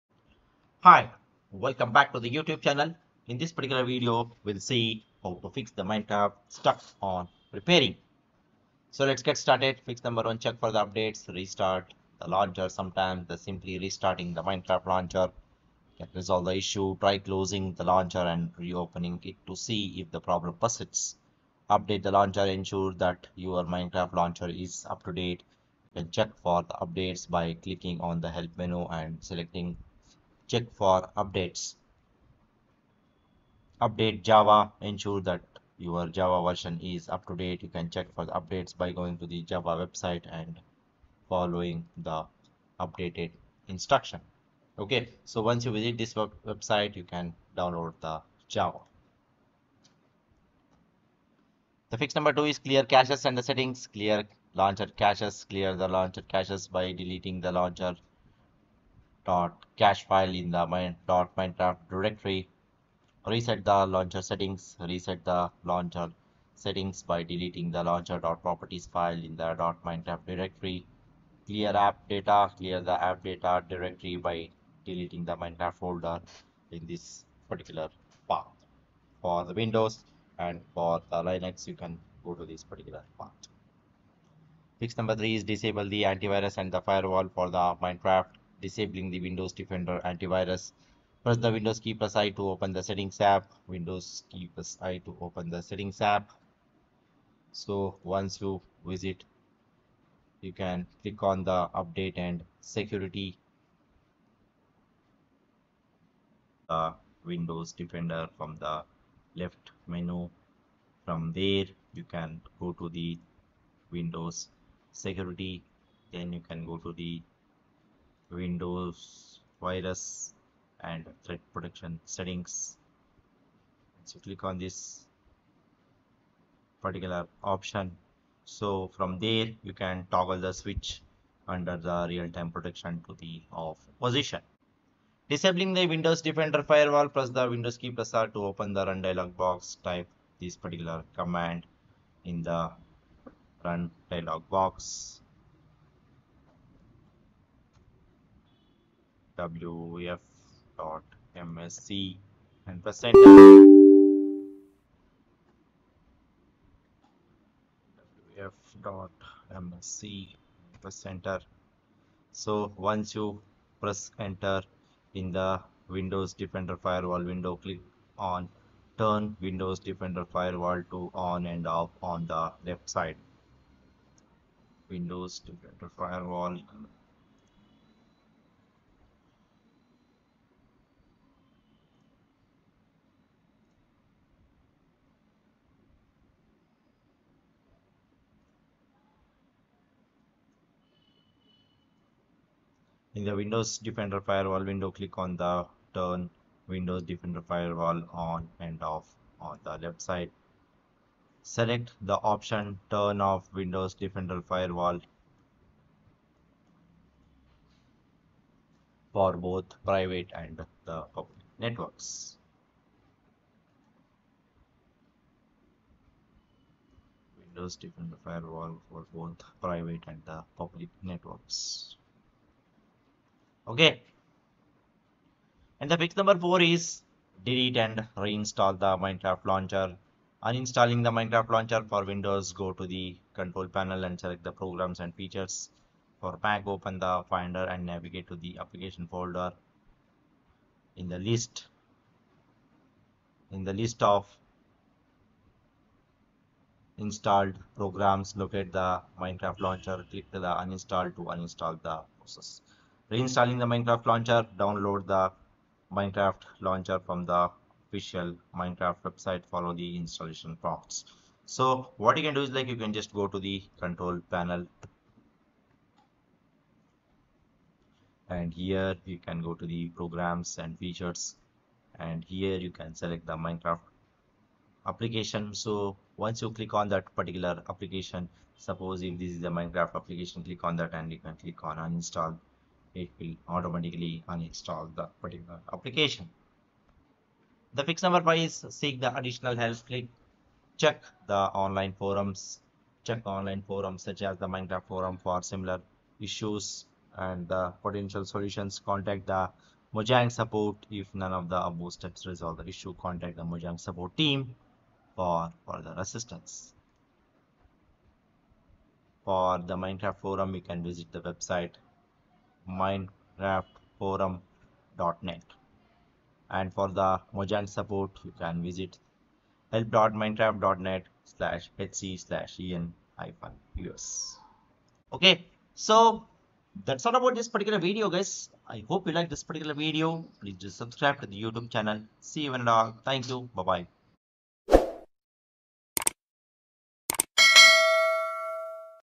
How To Fix Minecraft Stuck On Preparing
Today, we're going to show you How To Fix Minecraft Stuck On Preparing.This can be helpful in various situations, such as Fixing Minecraft's stuck on preparing issue ensures faster game loading, reduces player frustration, enhances gaming experience, and improves overall performance.

0:11 Fix 1. Check For Updates
0:52 Fix 2. Clear Cache And Settings
1:26 Fix 3. Disable Antivirus And Firewall For Minecraft
3:46 Fix 4. Delete And Reinstall The Minecraft Launcher
4:49 Fix 5. Seek Additional Help